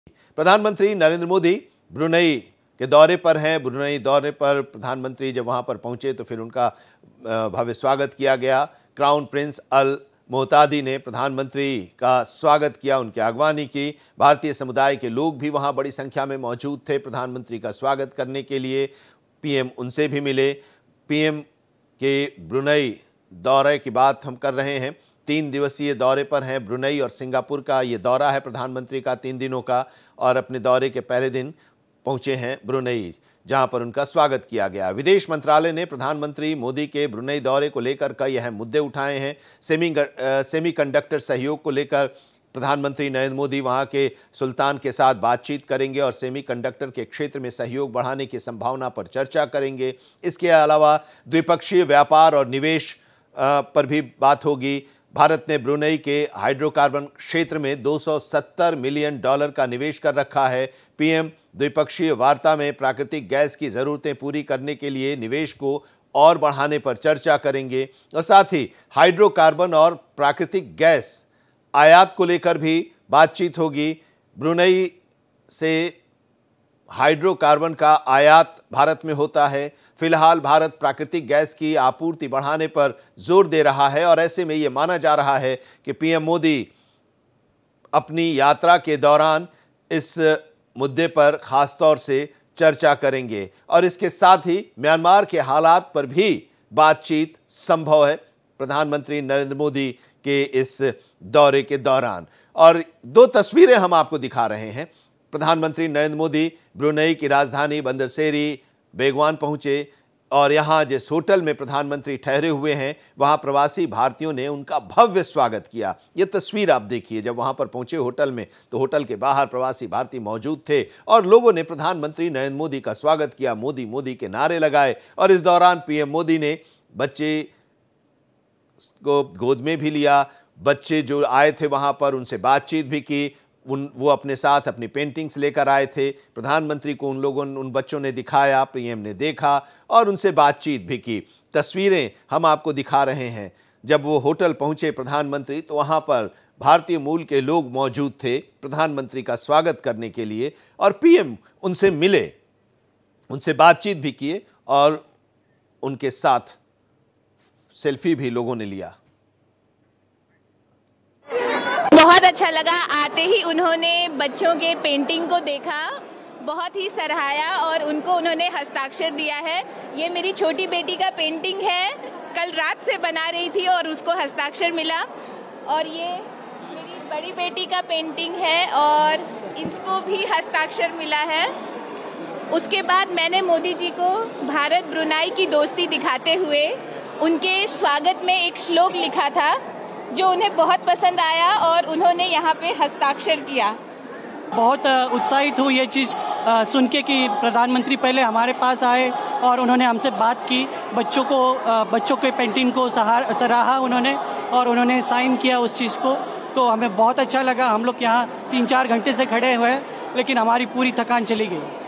PM Modi Brunei Visit: Brunei पहुंचे PM Modi, क्राउन प्रिंस ने किया स्वागत | BharatExpress
swati